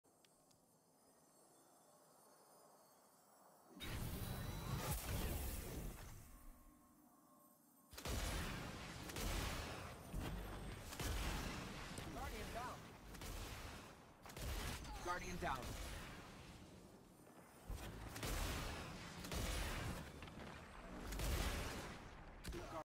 The New Dungeon LAUNCHER Has a Unique Interaction.
How to shoot 10 rockets without reloading rapidly